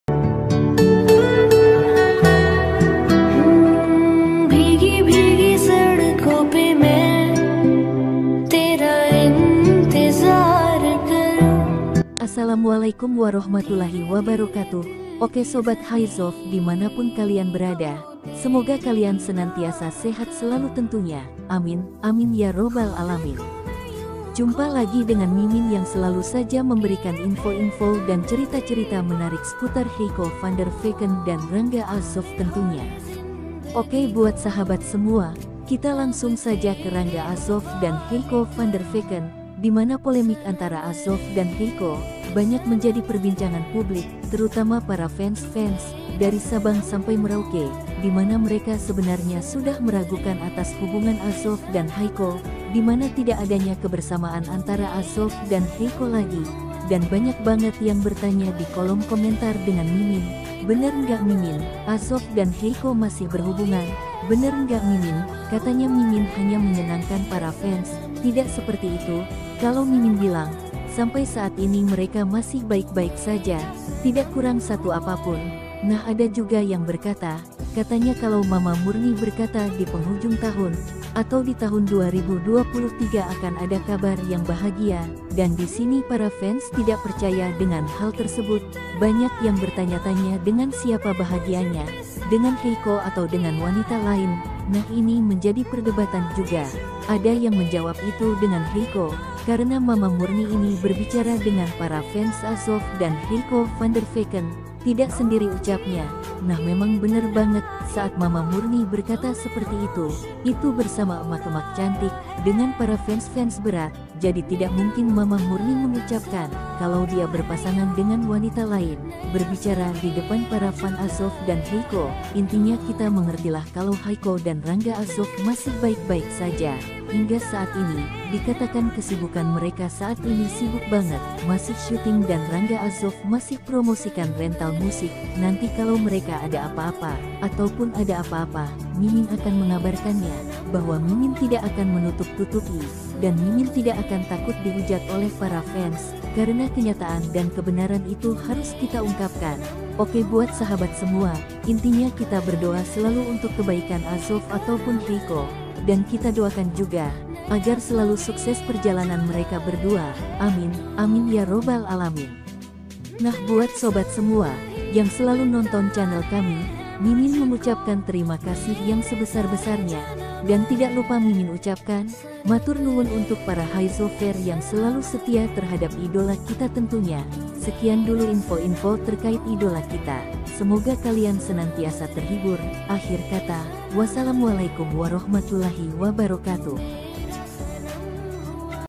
MAMA MURNI TETEP DENGAN PILIHAN NYA UNTUK PENDAMPING RANGGA AZOF
MAMA MURNI TETEP DENGAN PILIHAN NYA UNTUK PENDAMPING RANGGA AZOF //haico Van der veken dan Rangga azov

__Nama Artis muda Rangga Azof dan Haico Van Der Veken Kian Melambung tinggi  Setelah Mereka berdua Membintangi dan sekaligus Dipasangkan Di sinetron buku kariyan seorang istri.

Banyak sekali Fans Bucin & Kusut Ya ng berharap Agar Rangga Azof dan Haico berjodoh Didunia nyata.Banayak fans yg mengaku baper ( Bawa perasaan ) Setelah Melihat Cemistri keduanya di sinetron samudra cinta Hingga akhir nya mereka berduapun Dido'akan berjodoh.

__SEKIAN & TERIMAKASIH__